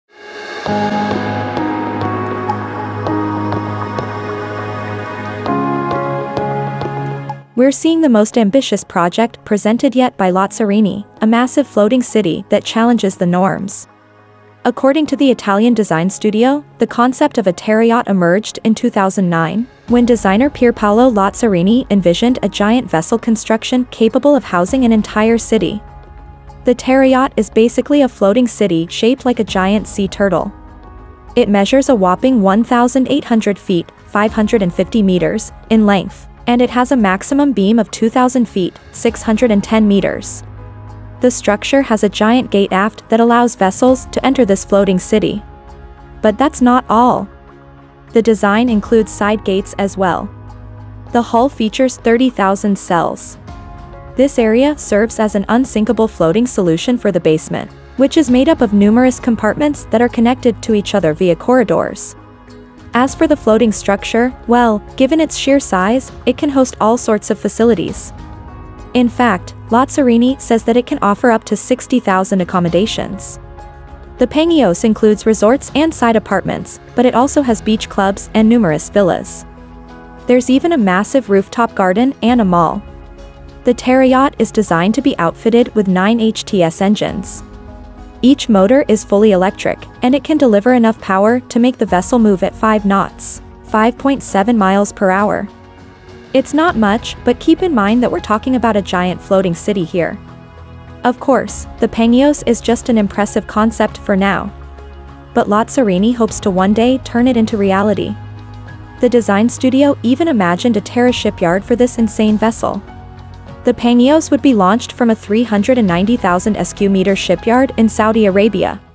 Floating City Looks Like a Giant Sea Turtle - Pangeos Terayacht
The terayacht is basically a floating city shaped like a giant sea turtle. It measures a whopping 550 meters in length, and it has a maximum beam of 610 meters.

Pangeos is just an impressive concept for now. But Lazzarini hopes to one day turn it into reality. The design studio even imagined a terashipyard for this insane vessel. The Pangeos would be launched from a 390,000 square meter shipyard.